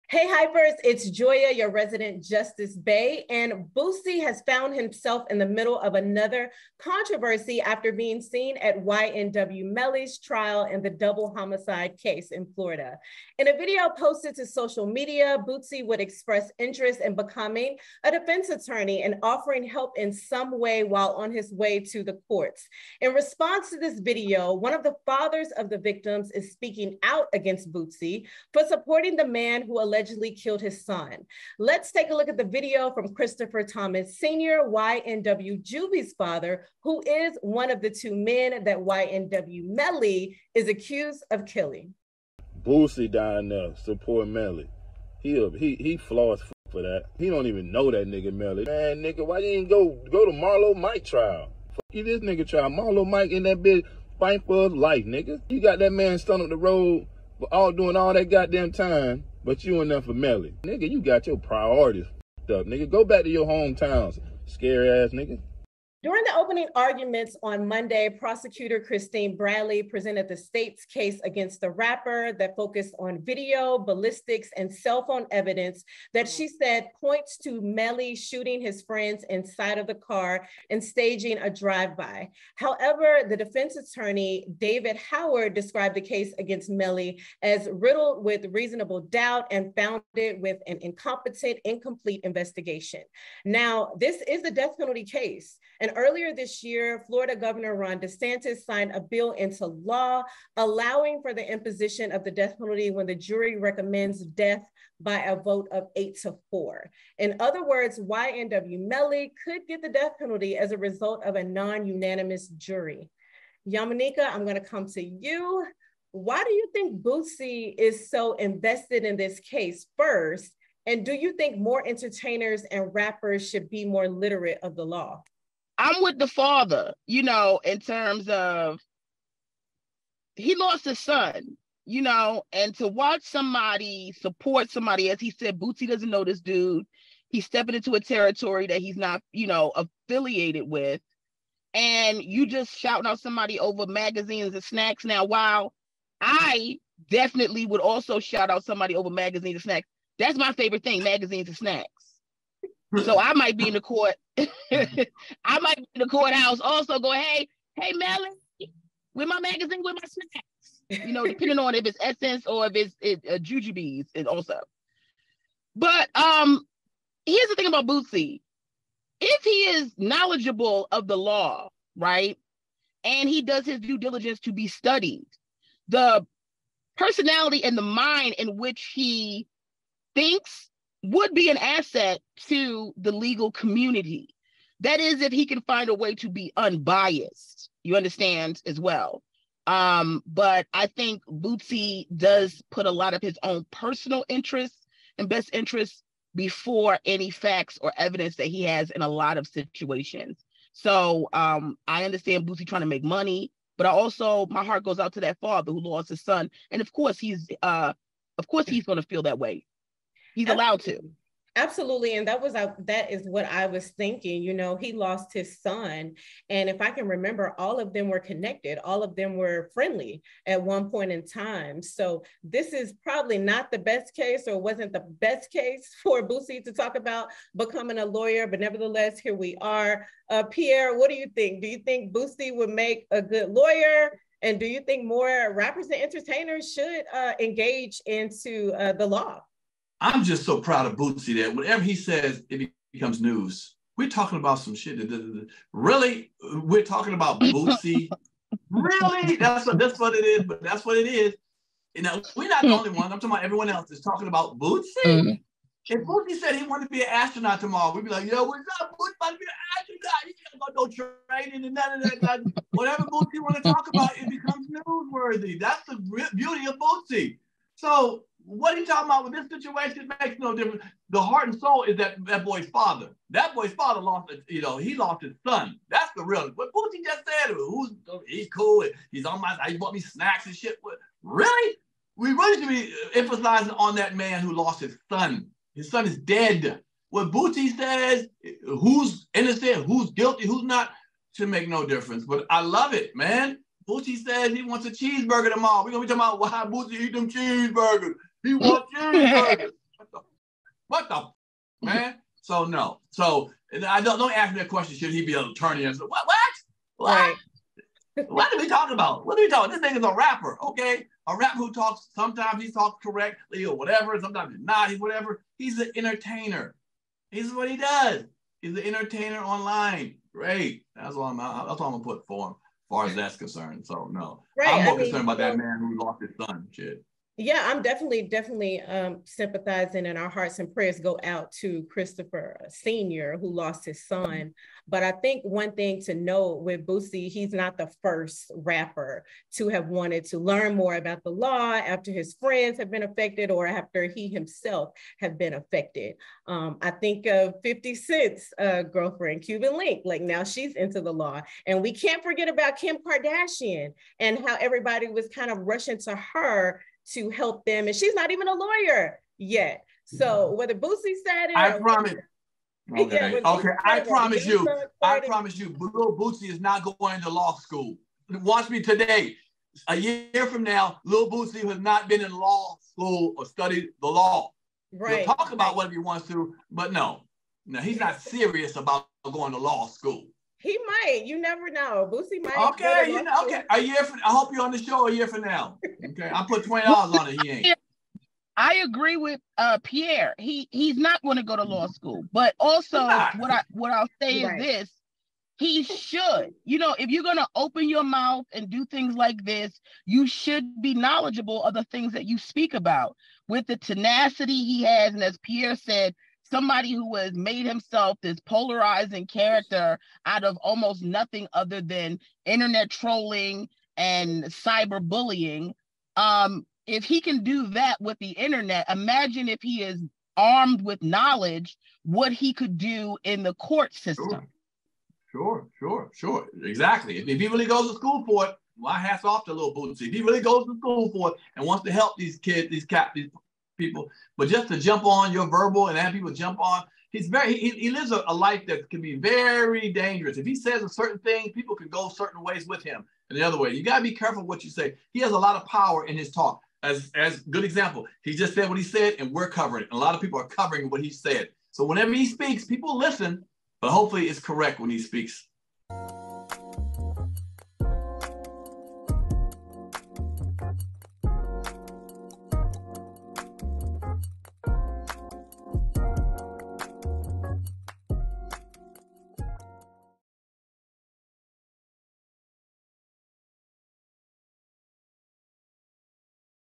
Angry Dad Checks Lil Boosie For Supporting YNW Melly Trial: "You Got Your Priorities F*cked Up"
Comedy Hype Live in Atlanta (June 27) A father in connection to the ongoing YNW Melly case has now spoken out. A clip would surface with the the father of YNW Juvy would take to social media with his concerns of Lil Boosie showing support towards the current defendant. Instagram: @ComedyHype & Twitter: @ComedyHype_